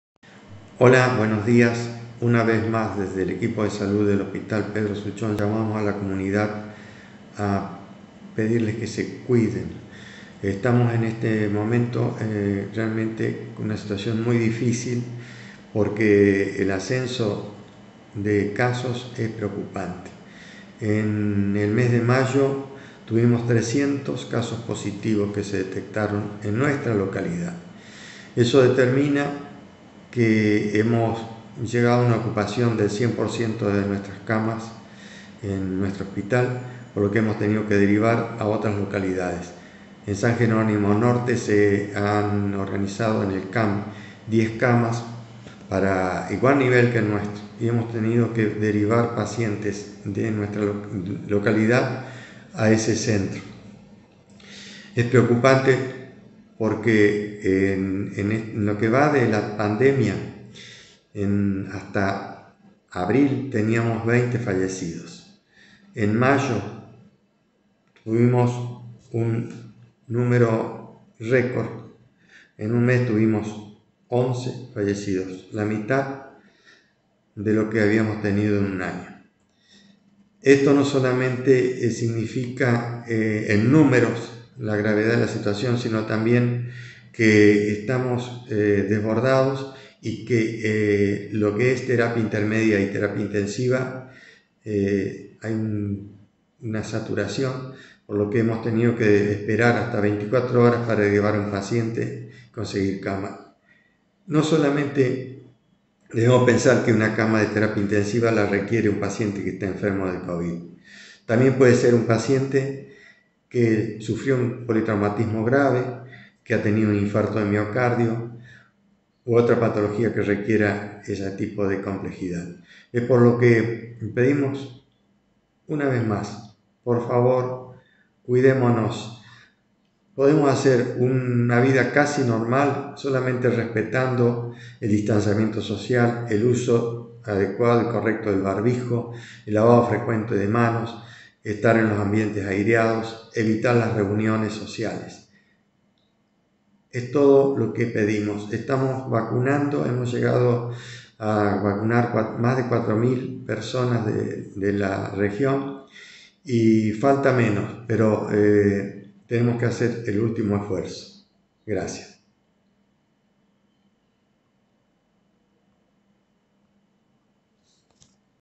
COVID EN MAYO: 300 CASOS POSITIVOS, 100% DE OCUPACIÓN Y 11 FALLECIDOS
Así lo expresó el Médico Director del Hospital Suchón Luis Escobar en otro mensaje que grabó para que la comunidad sancarlina tome conciencia de la situación actual en cuanto a la pandemia de Covid - 19
Escobar brindó algunos datos, solo del mes de Mayo; y dijo que hubo 300 casos positivos, lo que significa que se llegó a una ocupación del 100% de las camas del hospital.
Otro dato frío, pero no por eso menos importante para tomar conciencia, es que desde que inició la pandemia hasta el mes de abril había 20 fallecidos; y en el mes de Mayo hubo un número récord de 11 fallecidos en tan solo un mes, la mitad de lo que hubo en un año.
El director del Hospital volvió a pedir que nos cuidemos, que seamos conscientes, que tomemos la RESPONSABILIDAD que la situación amerita.
Debemos hacerlo por cada uno de nosotros, de nuestras familias, amigos y también por el RESPETO que se merecen los profesionales de la salud.
“Se llevan más de 4.000 vacunas colocadas, falta menos, pero hay que seguir cuidándose” dijo Luis Escobar
POR FAVOR, desde Datasancarlos.com te pedimos que escuches este mensaje completo y lo compartas.